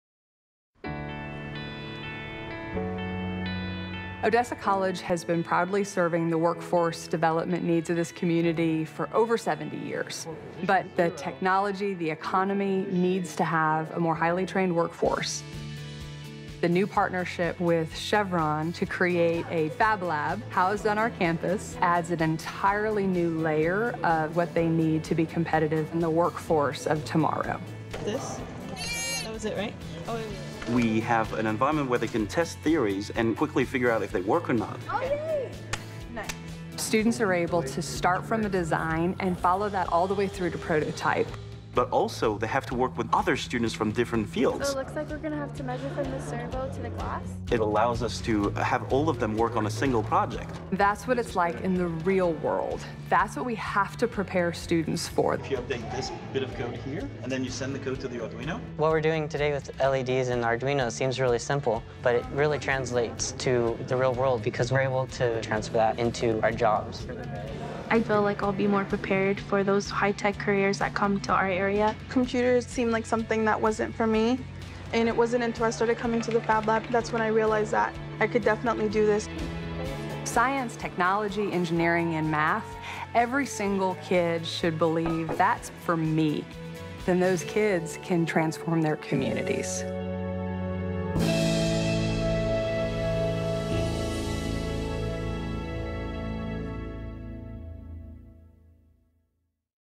Workforce: Fab Lab Permian Basin at Odessa College
The Fab Lab Permian Basin and mobile Fab Lab opened at Odessa College in February 2017 to provide students and aspiring inventors in the region the resources to create, experiment and build their skills in STEM. The Fab Lab also provides workforce development education programs to  increase the skilled workforce in the region. Students have the opportunity to use a suite of digital fabrication tools and prototyping machines such as laser cutters, 3-D printers, vinyl cutters and milling machines, used for making and designing.